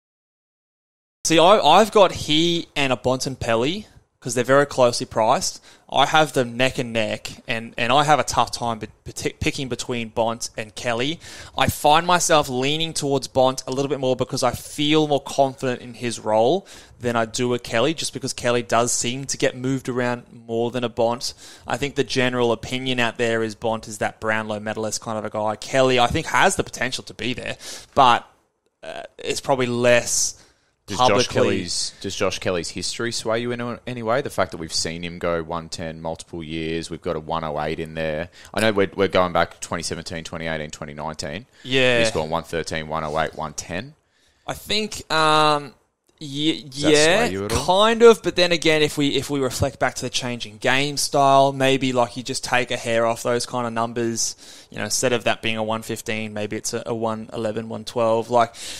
Marcus Bontempelli vs Josh Kelly
Mitch and Luke dicsuss Bont vs Jelly as POD options for your AFL fantasy team.

Follow the boys on Twitter!

Mitch - @ballboysfantasy

Luke - @lukerogo17